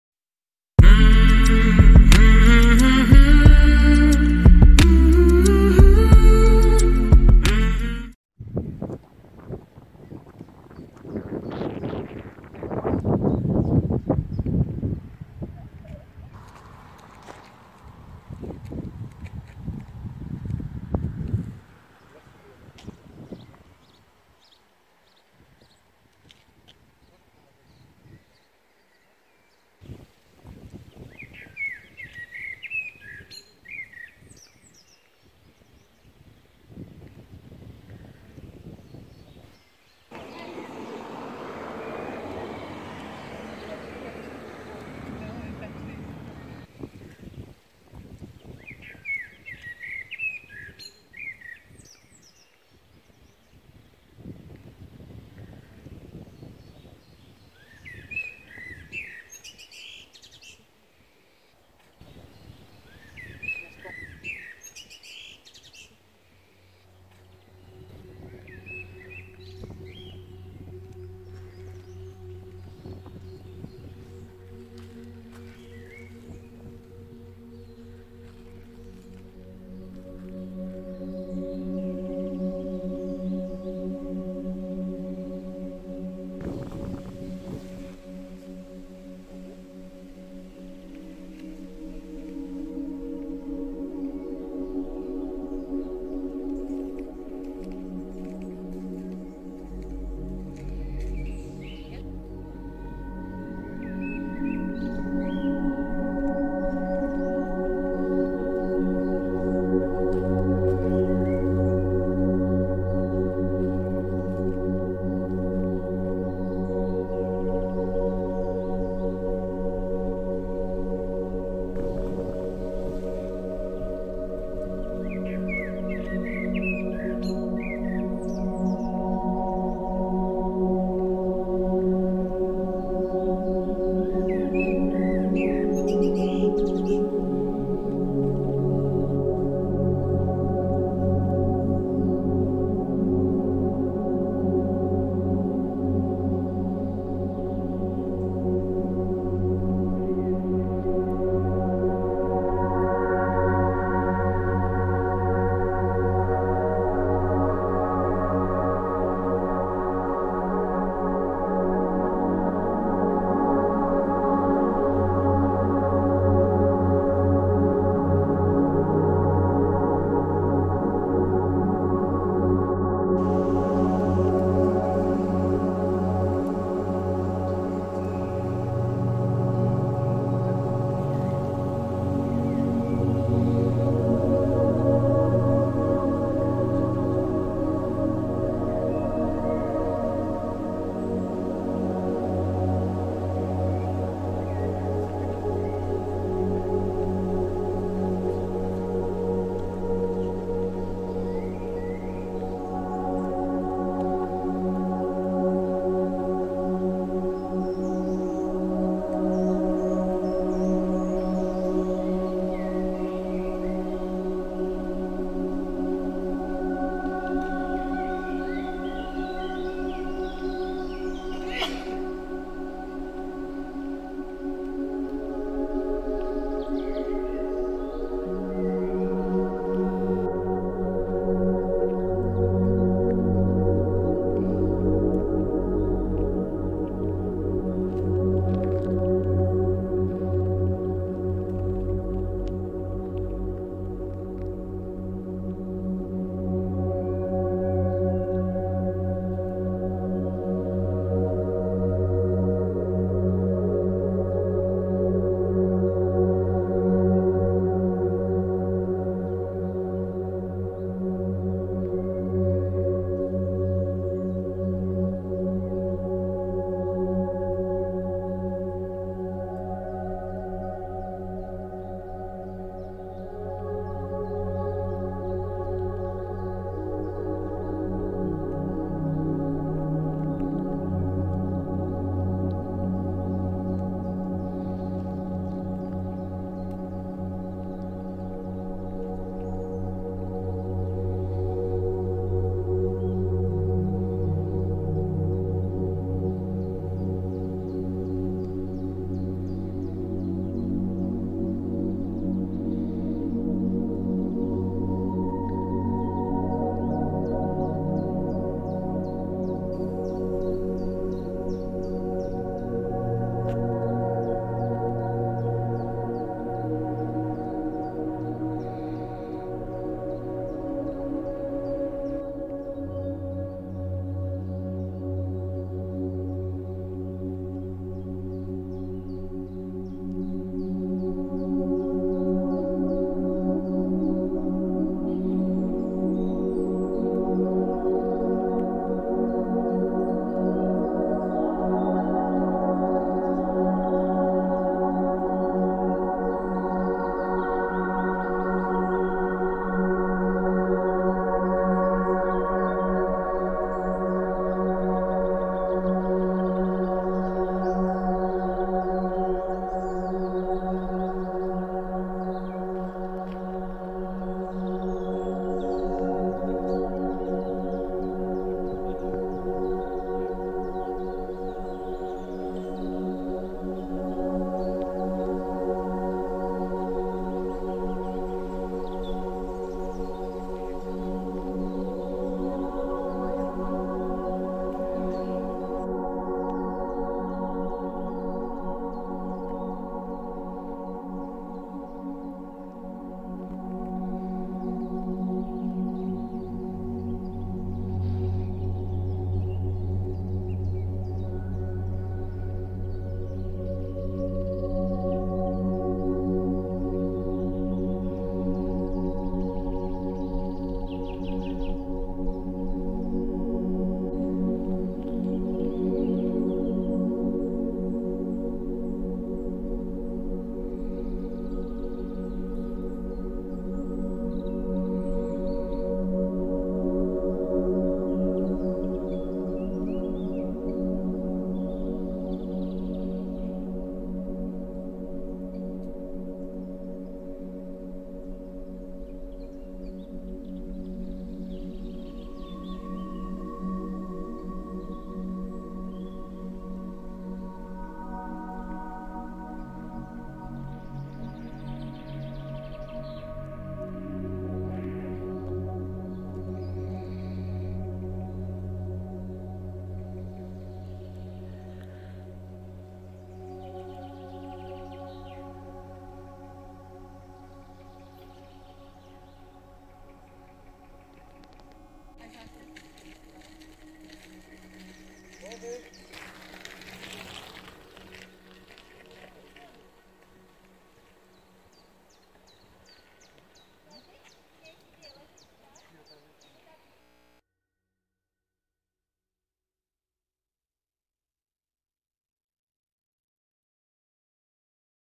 Просто прогуляемся по лесу
Воскресная прогулка по весеннему лесу.

Sunday walk in the spring forest.

Sonntagsspaziergang im Frühlingswald.